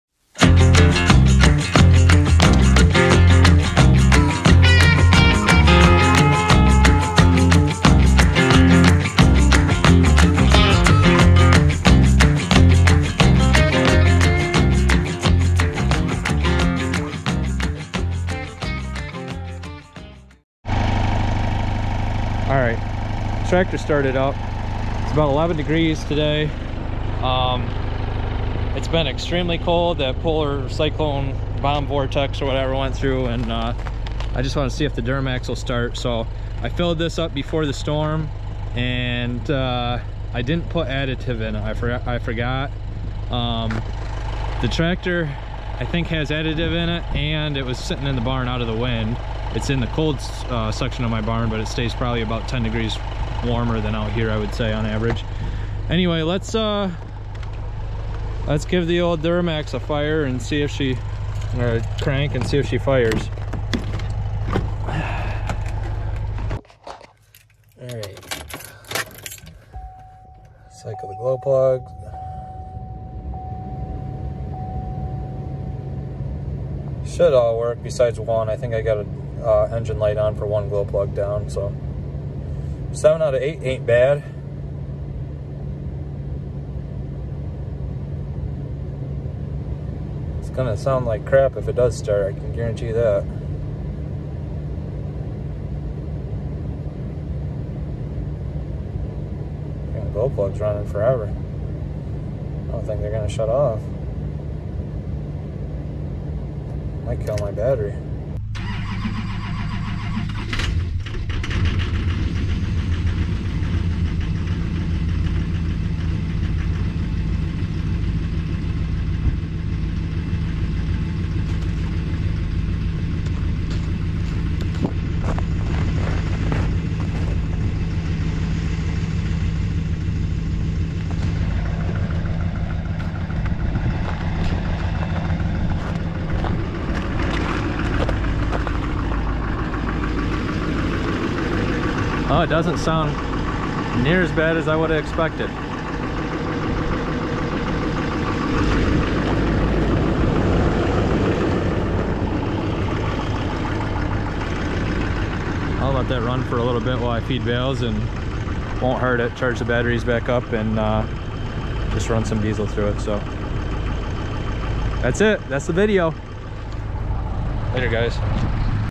Duramax Cold Start After Two Days of -20F Windchill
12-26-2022. Starting the duramax the day after the polar bear cyclone bomb vortex of the century of 2022 went through.